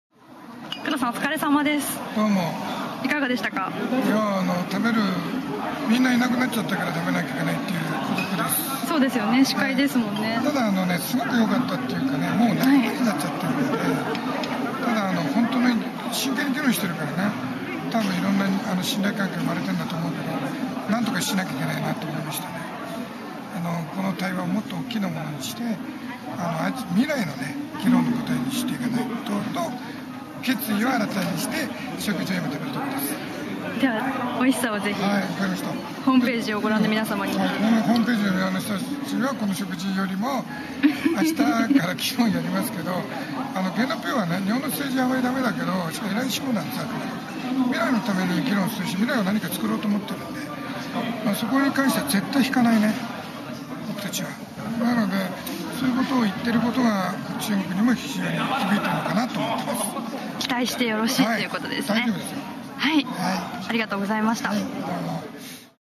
晩餐会後の工藤にインタビュー
工藤が晩餐会の感想、明日以降の意気込みを語ります。